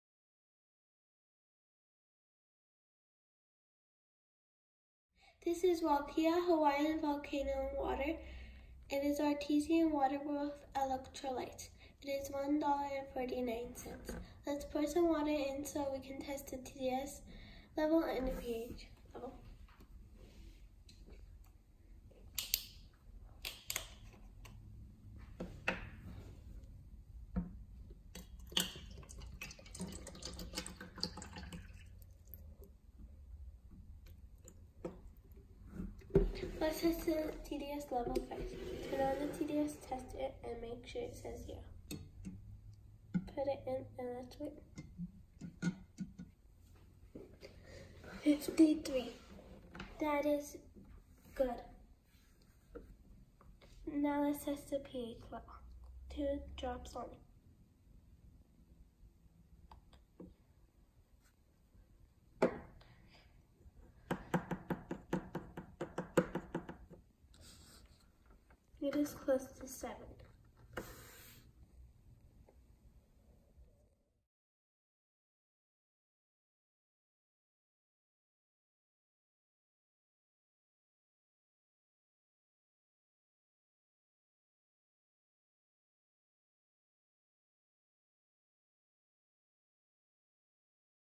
Waiakea Water test - pH and TDS
Waiākea Hawaiian Volcanic Water
If you'd like to buy this water from Amazon, please

Results:
pH: 7.5
TDS: 53